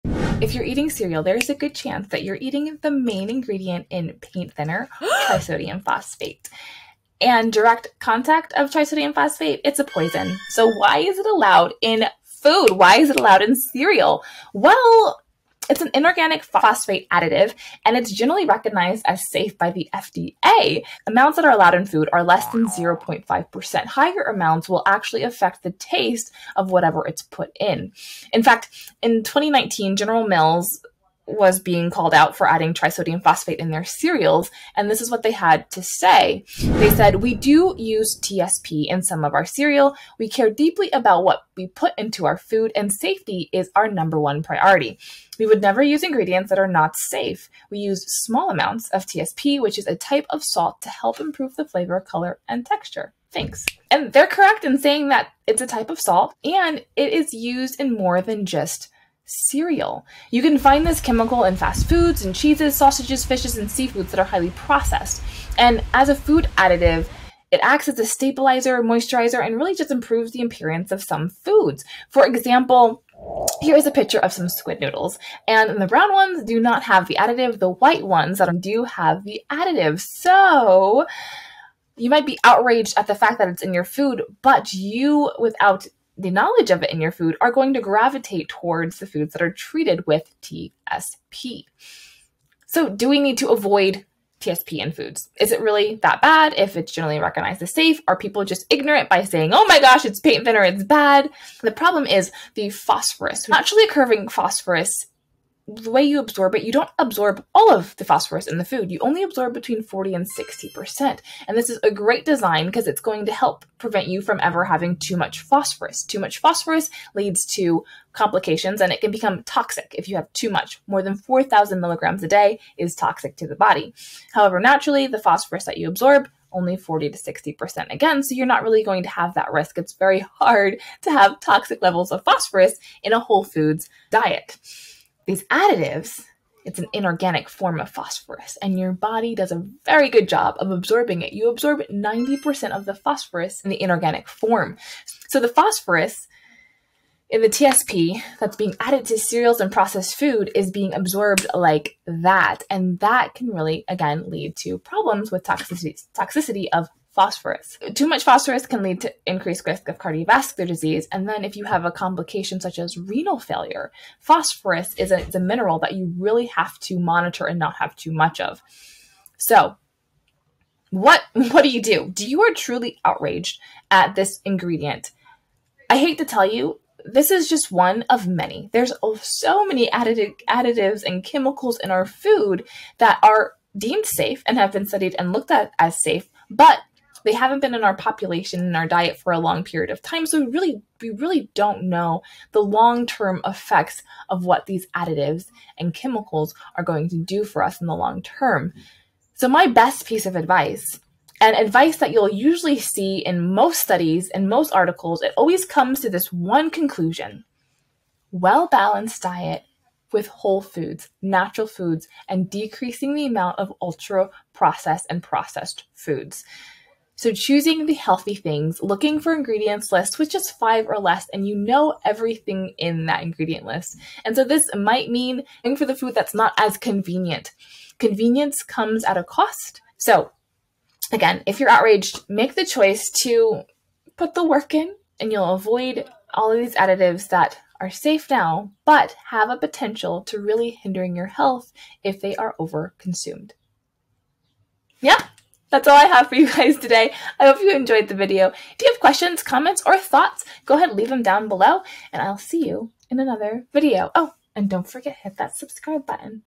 Trisodium Phosphate aka Paint thinner is in more than just your Cereal!
Trisodium Phosphate, the main ingredient in paint thinner has made its way into our food!
Why are they putting this in foods like cereals and other packaged foods? Watch the video to find out.